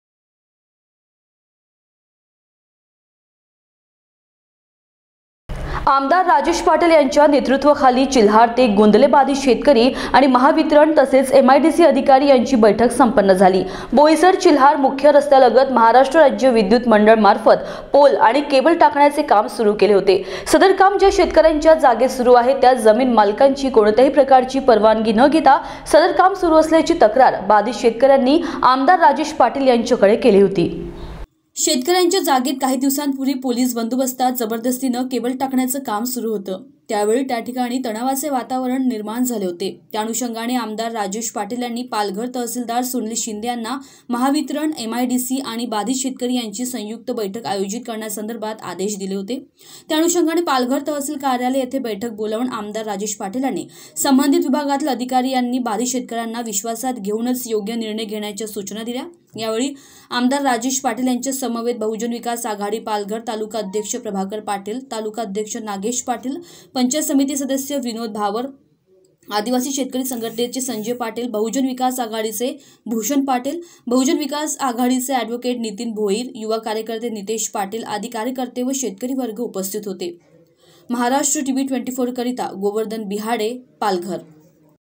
महावितरण, एम आय डी सी,आणि बाधित शेतकरी यांची संयुक्त बैठक | 18 jan 2022
आ. राजेश पाटील यांच्या नेतृत्वाखाली बैठक संपन्न